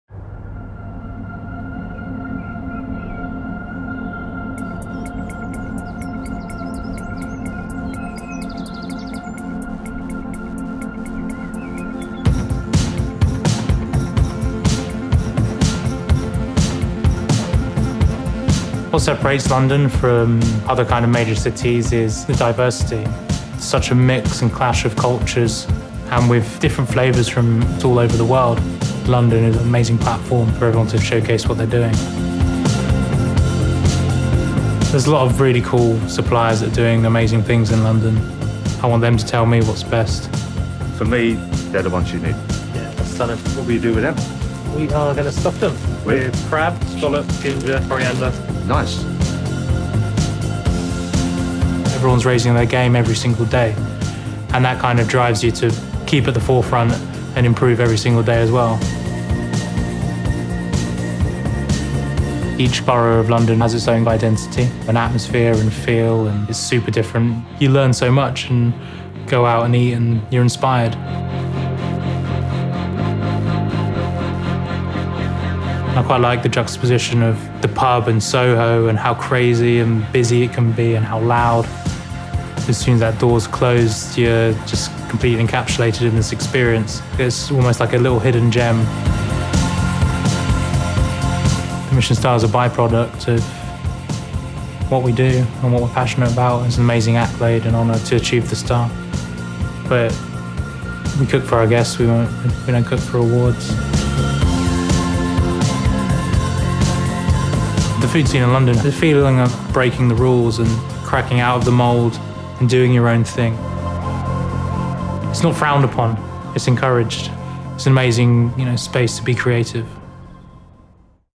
Dawn Till Dusk: London with the Selby Brothers | Expedia
We follow Luke and Theo Selby, award-winning chefs of Evelyn’s Table, Soho, on a culinary tour of London.